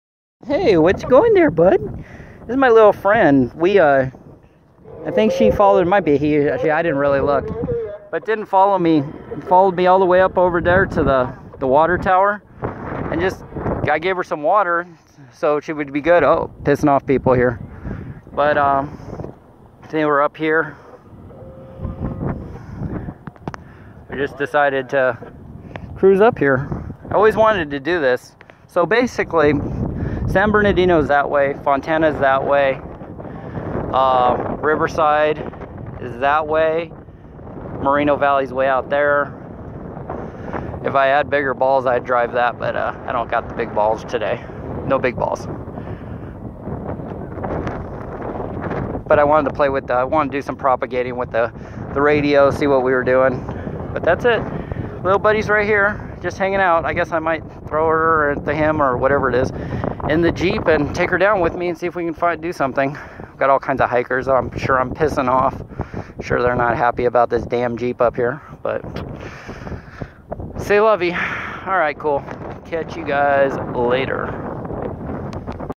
Just out making friends
All the attention that I was giving to the motorcycle the jeep was getting jealous. So I figured I'd go ahead and drive up here and play with my CB radio. As I'm uploading this video the pope is just laying next to me on the ground now what do I do.